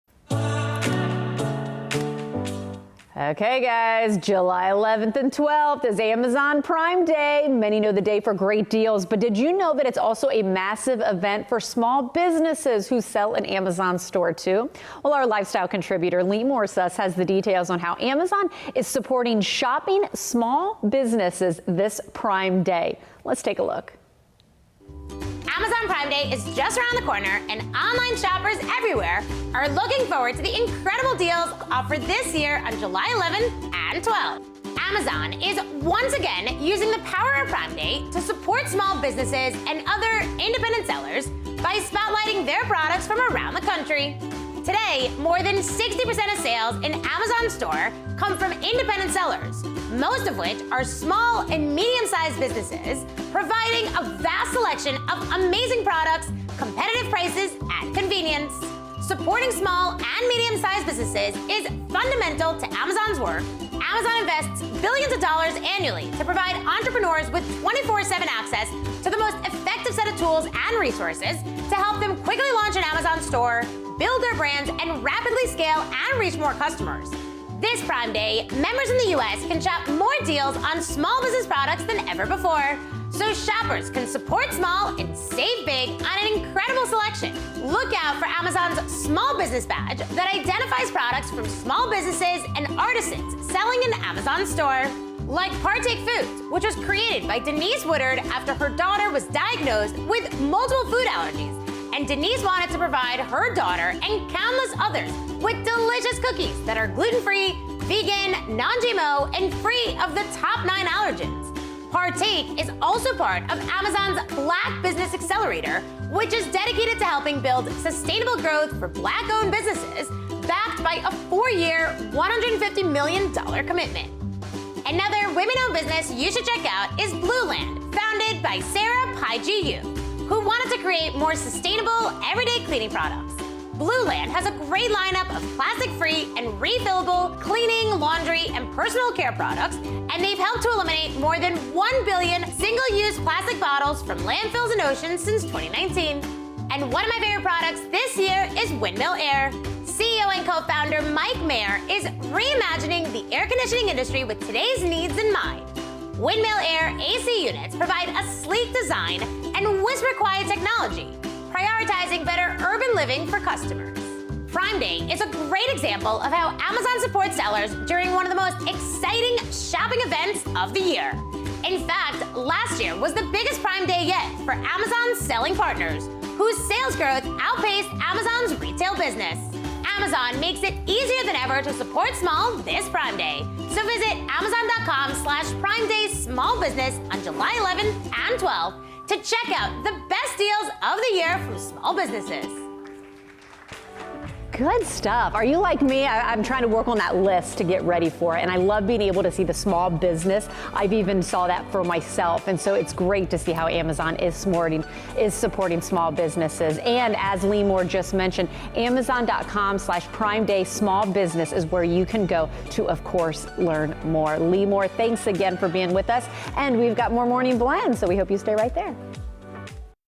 How You Can Support Small Businesses During Amazon’s Prime Day
During Prime Day, and all year long, Amazon supports small businesses and the unique products that they offer in its store.

Customers can shop some of the best deals from small businesses at Amazon.com/PrimeDaySmallBusiness.